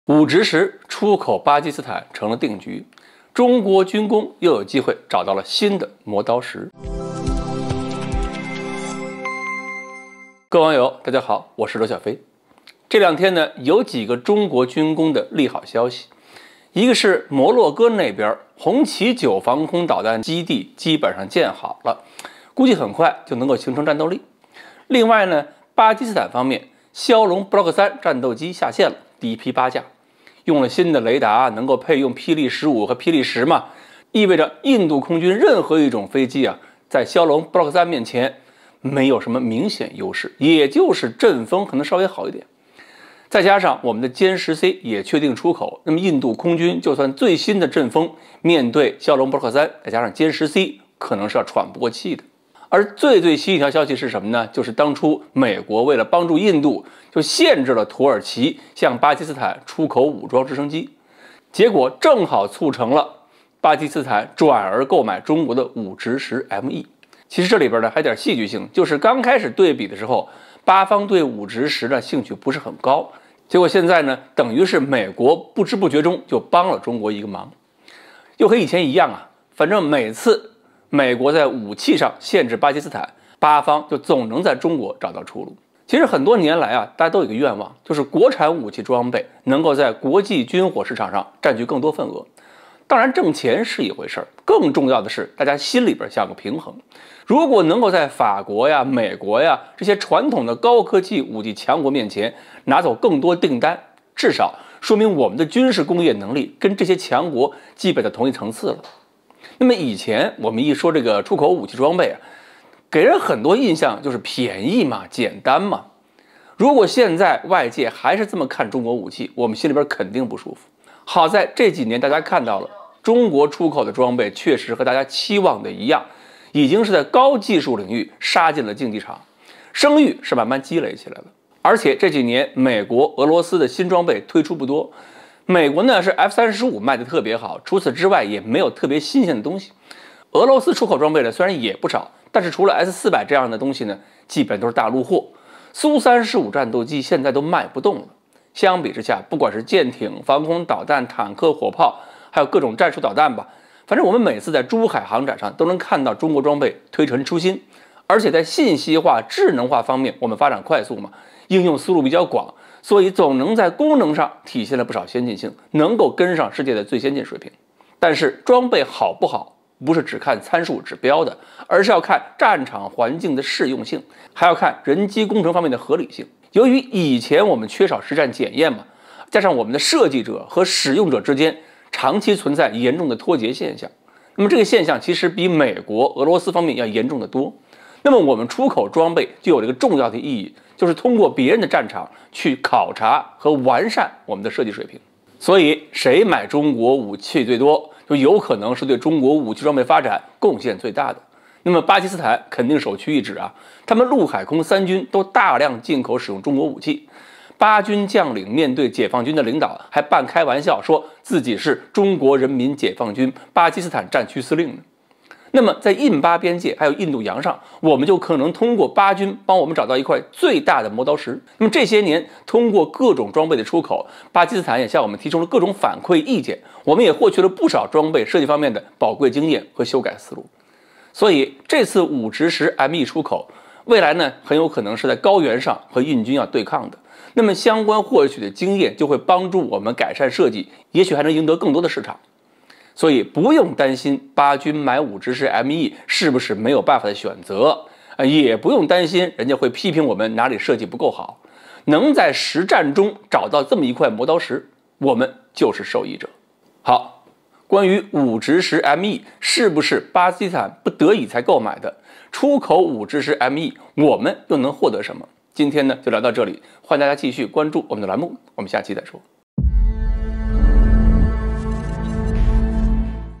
武直10成功出口巴基斯坦，美國幫了大忙，中國軍工找到新磨刀石！【刘晓非】
武直10出口巴基斯坦成定局，中國軍工又有機會找到新的磨刀石
武直10成功出口巴基斯坦，美國幫了大忙，中國軍工找到新磨刀石！